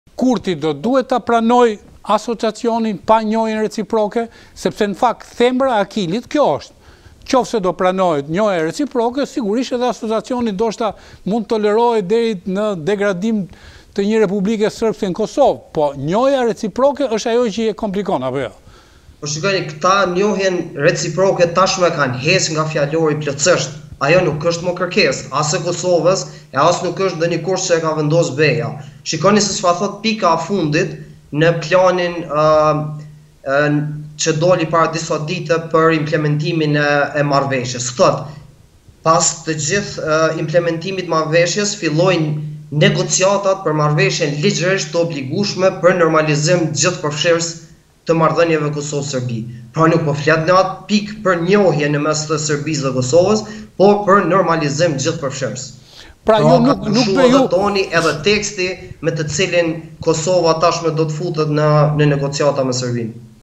Kurti: Në Bruksel u ekspozova para një skenari/ Ja cili është qëndrimi i PSD
𝐀𝐮𝐭𝐨𝐫 : NTV